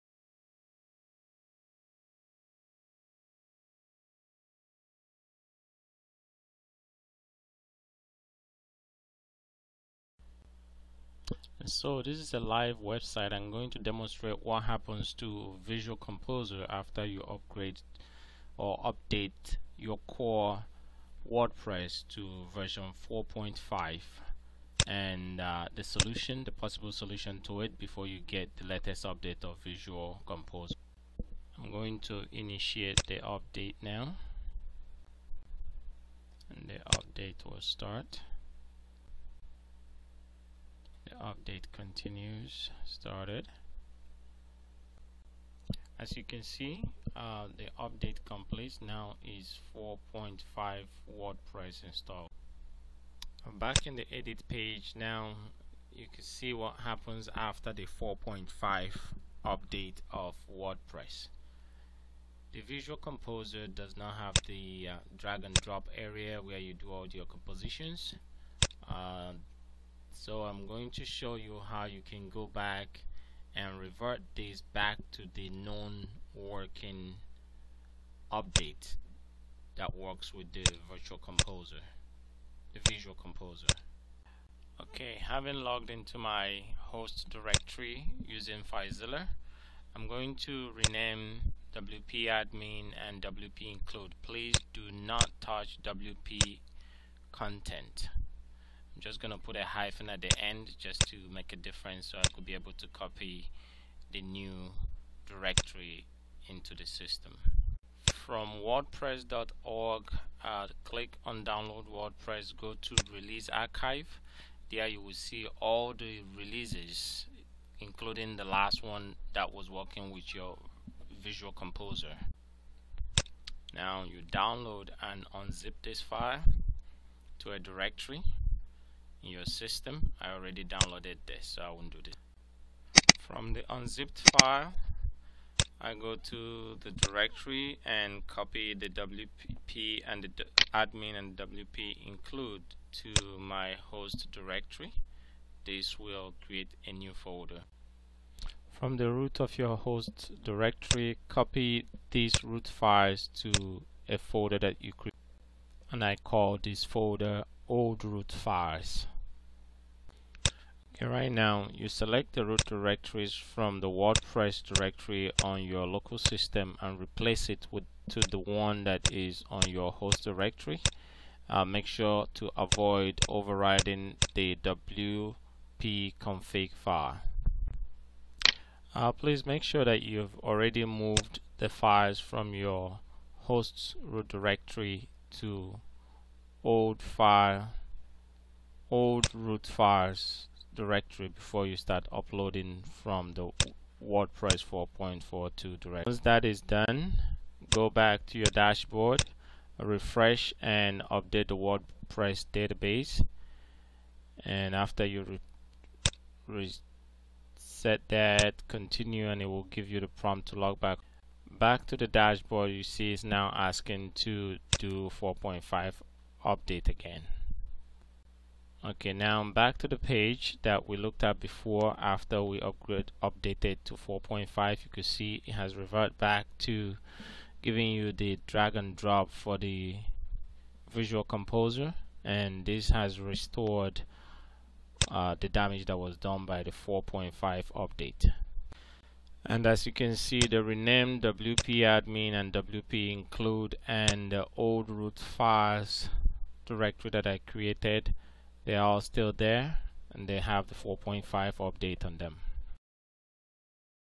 WordPress 4 5 and Visual Composer 4 9 Issues
How to revert back to the last known working WordPress update after WordPress 4.5 update